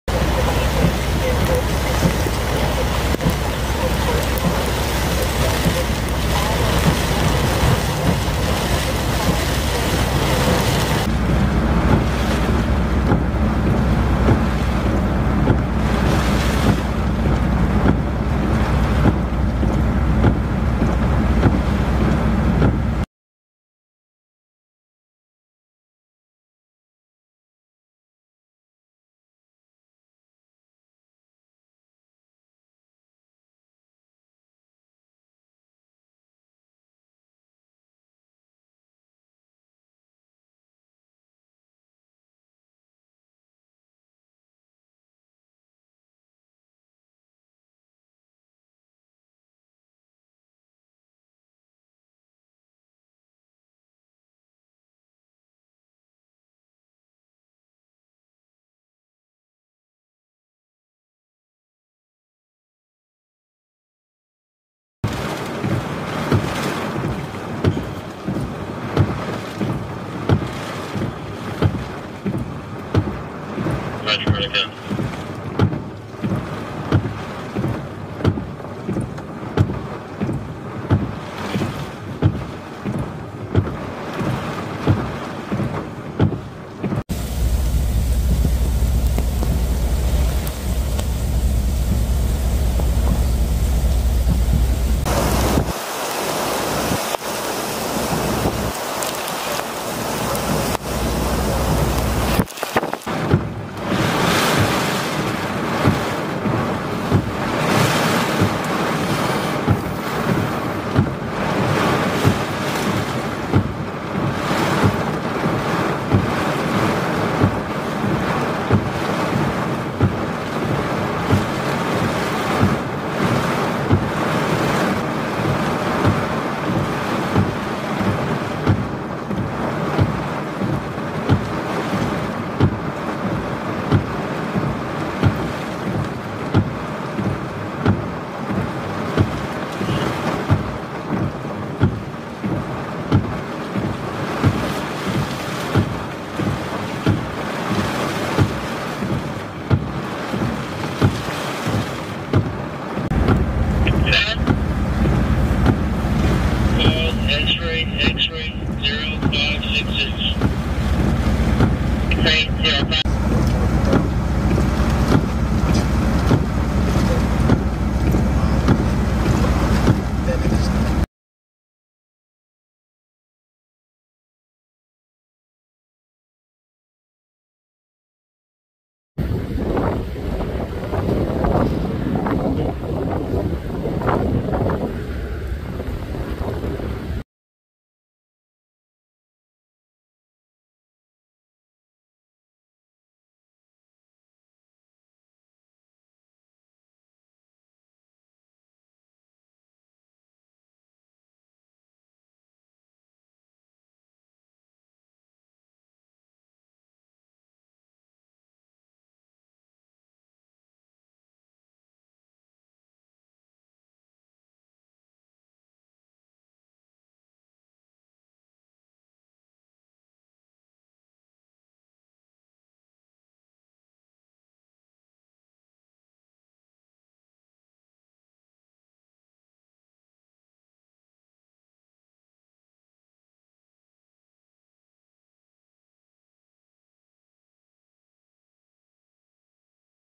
06-19-2024 Rockport, TX - Locals fight flood waters to get home before even more rain arrives
Wednesday after locals risk feet of flood water and surge to make it home before even more heavy rains arrived to Rockports Key Allegro neighborhood